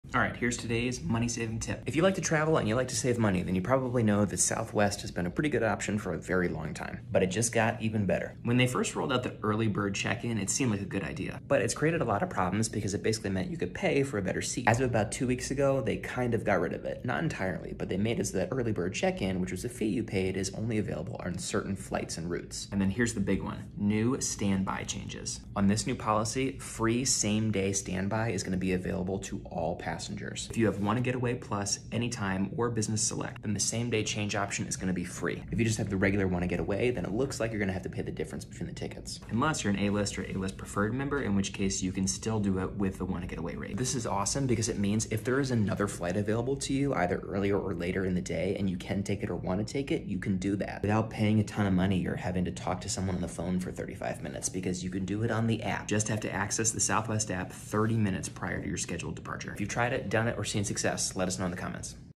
Save Money with Southwest Changes
Big fan of Southwest Airlines? Looks like last-minute travel changes just got a little bit cheaper! Learn about their new same-day standby policy with today's money-saving tip.